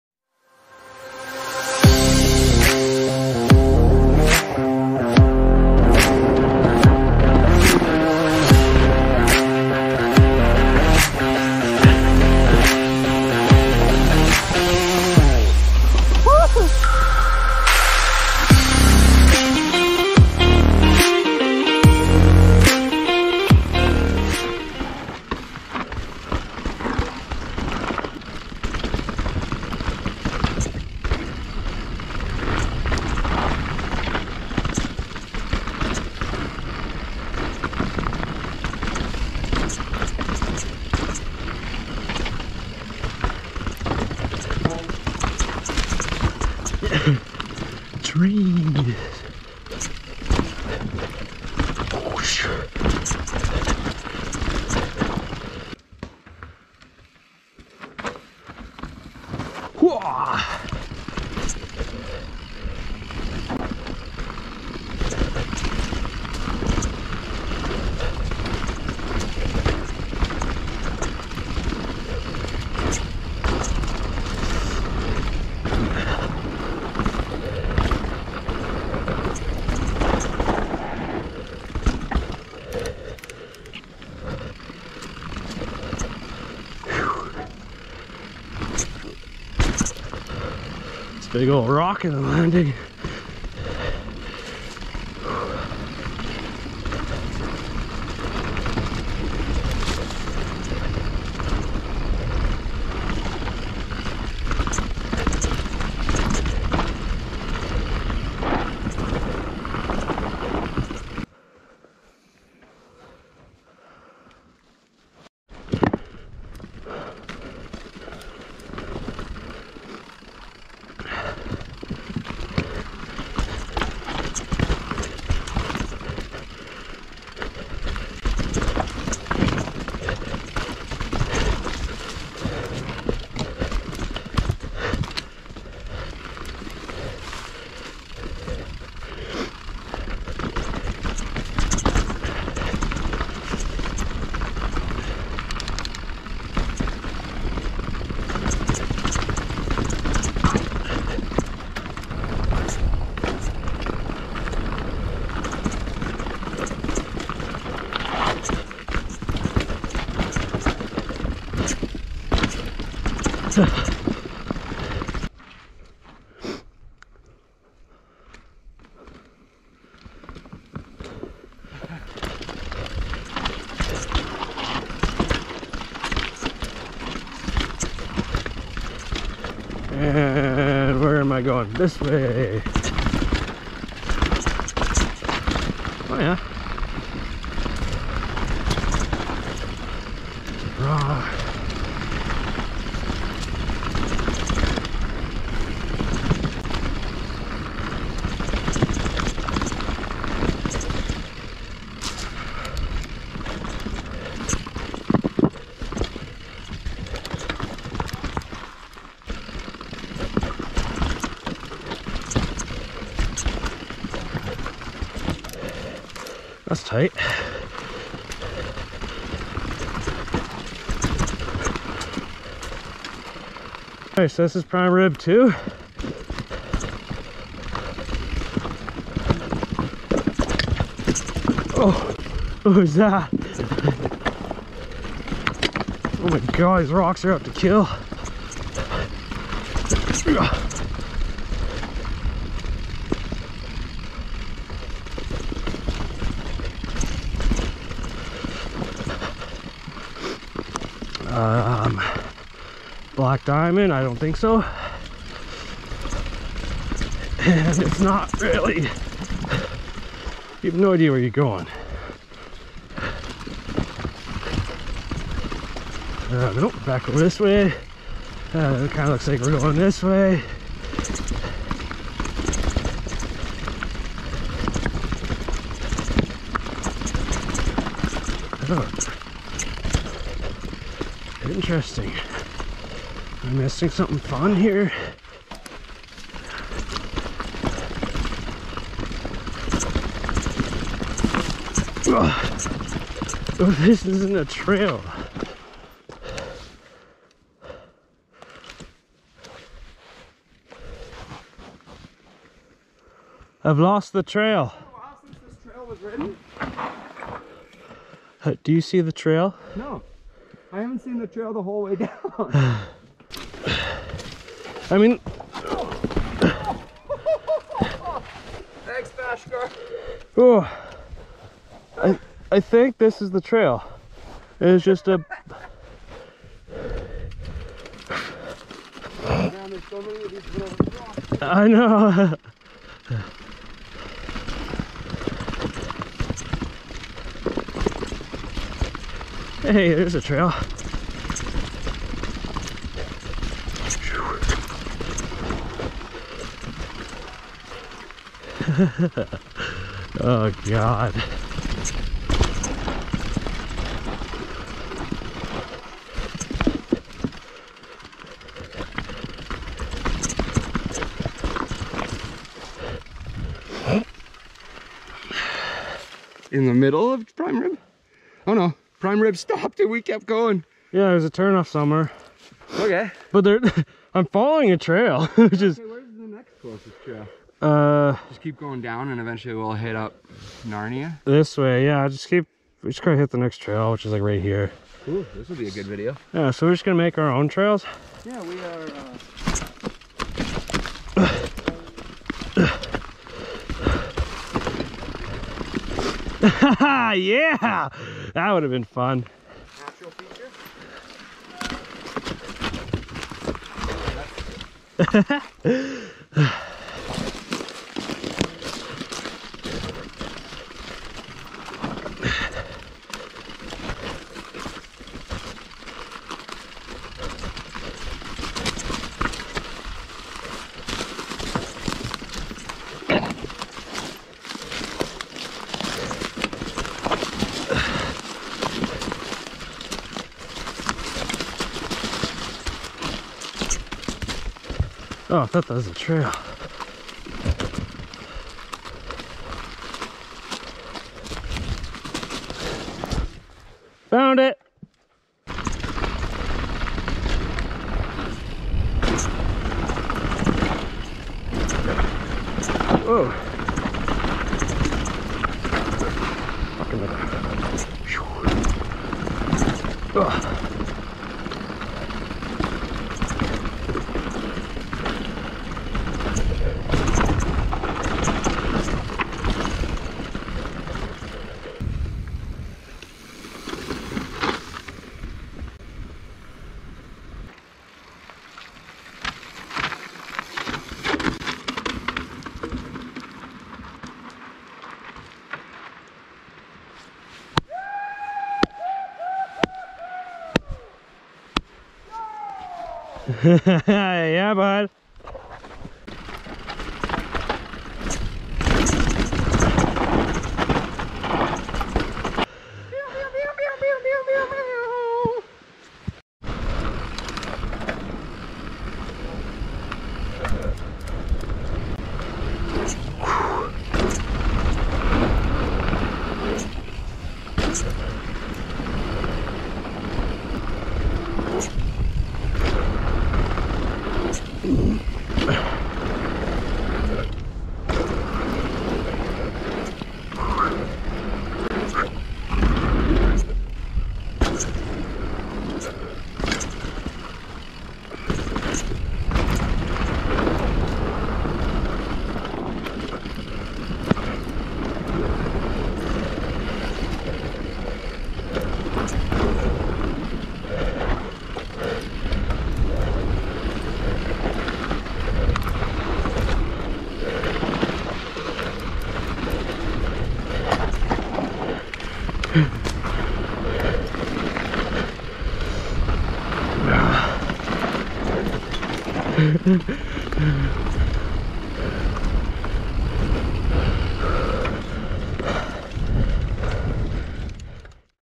ZACH & Q WENT UP A HILL
Q Conquered his first double black feature
we also tryed to do prime rib but got lost. again..